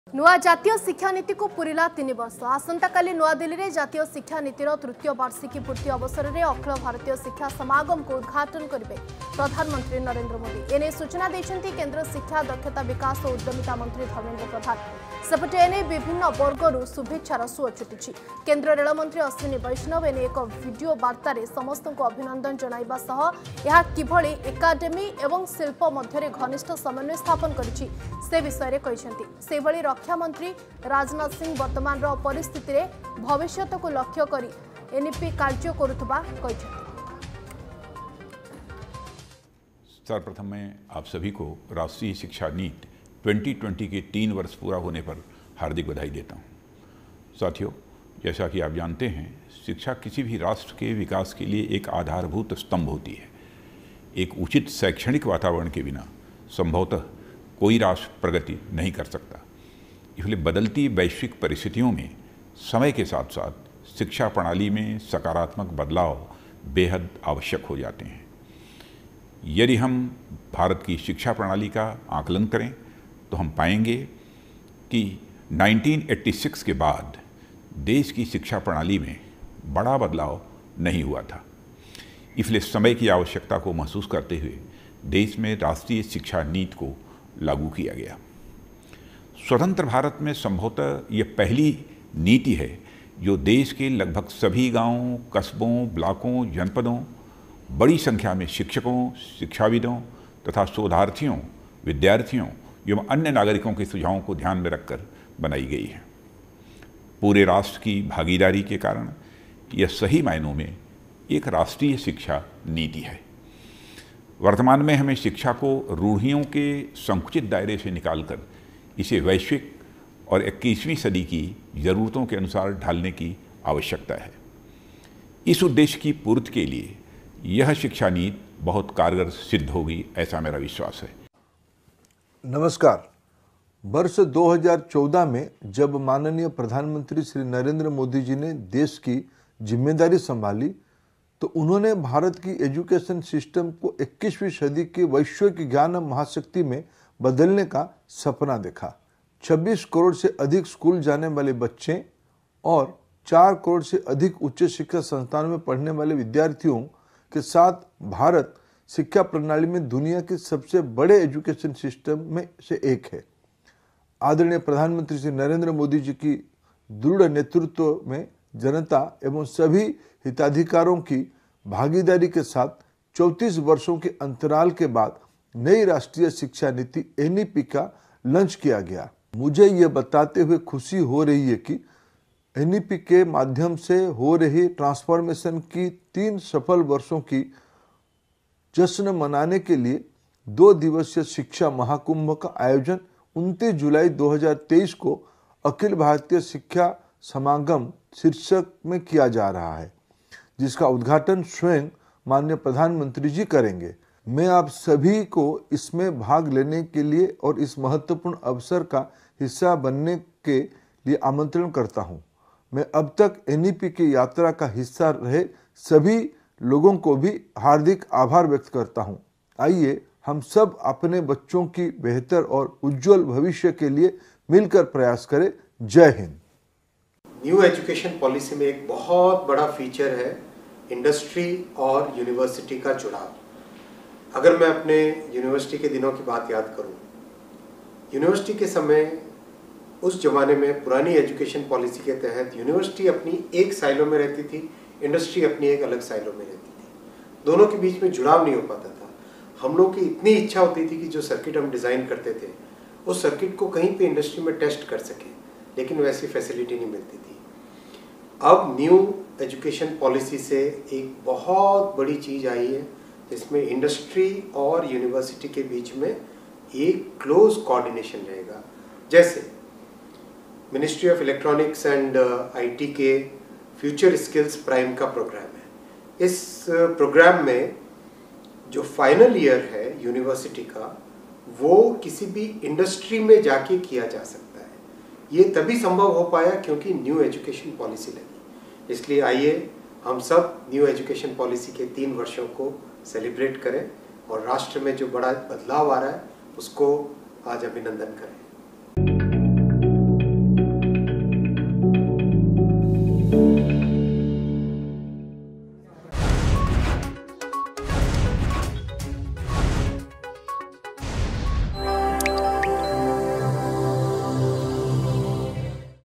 3 Years Of NEP, PM Modi To Inaugurate Akhil Bhartiya Shiksha Samagam Tomorrow In New Delhi
3 Years Of NEP, PM Modi To Inaugurate Akhil Bhartiya Shiksha Samagam Tomorrow In New Delhi